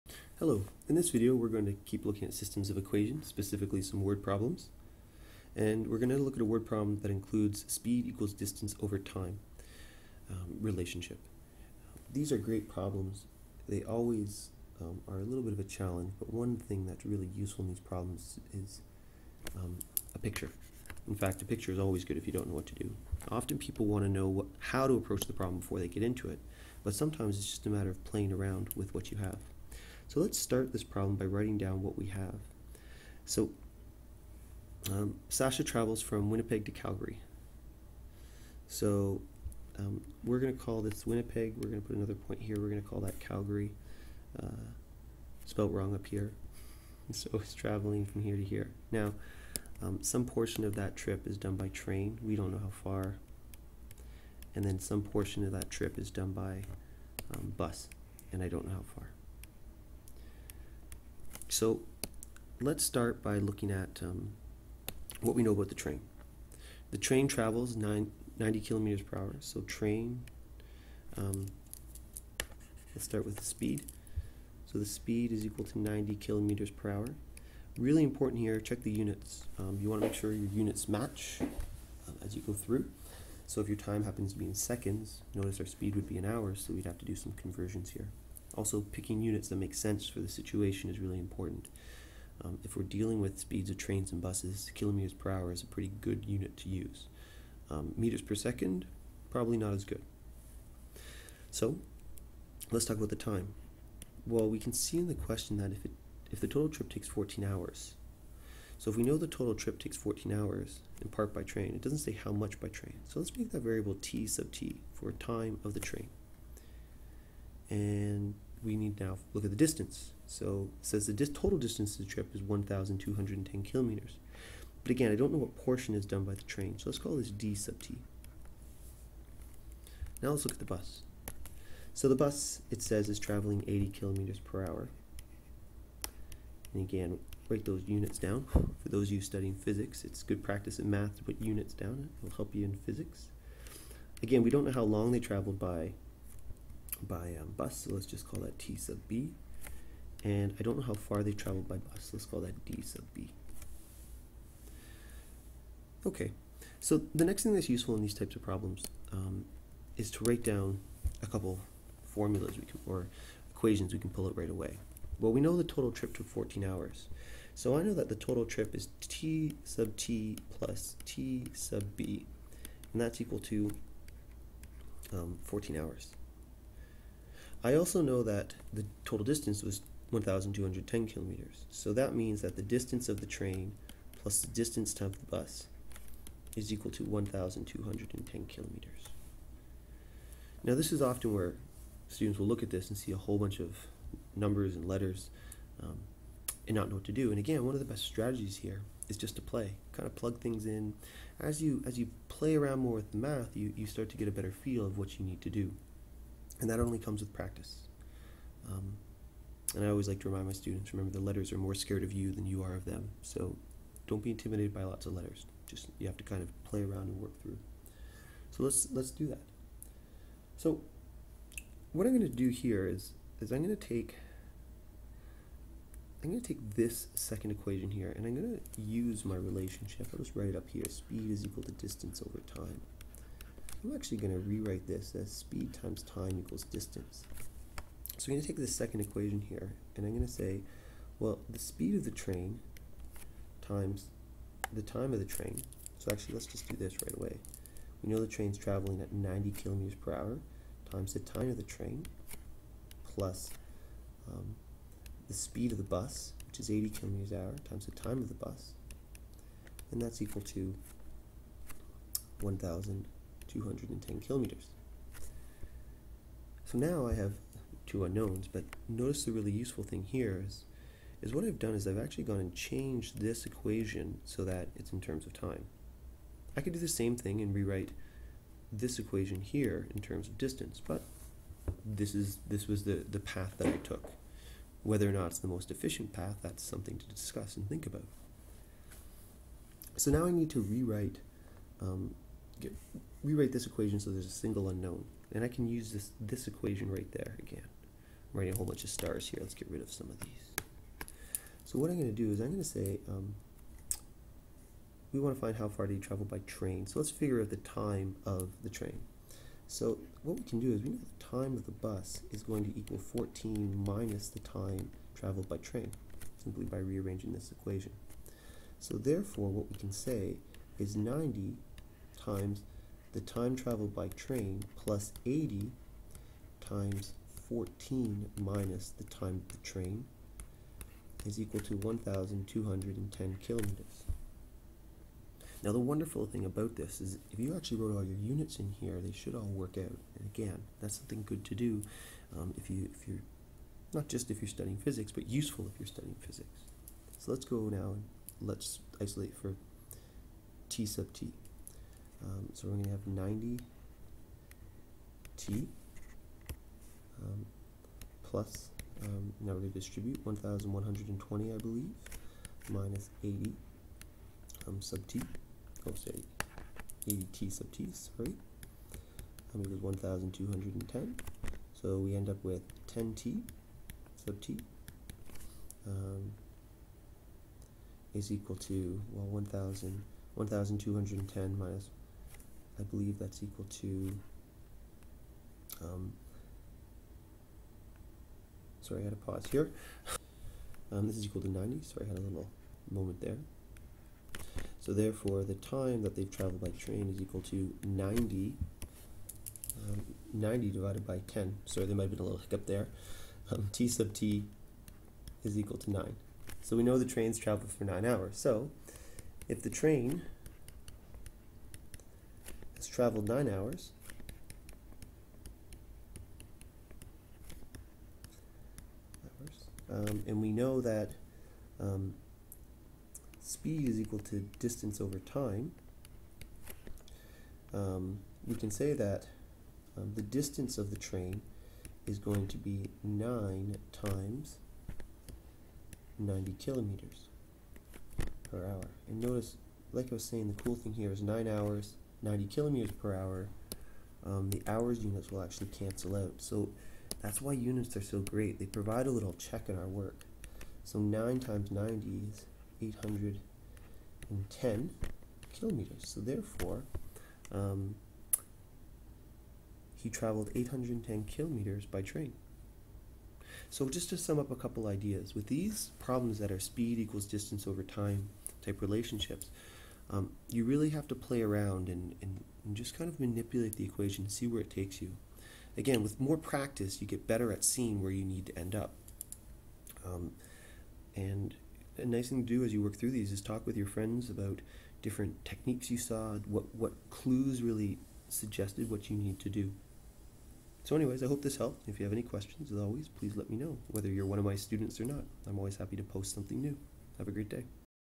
System of Equations - Word Problem (speed distance time)
THis video goes through how to solve a word problem incorporating speed distance and time.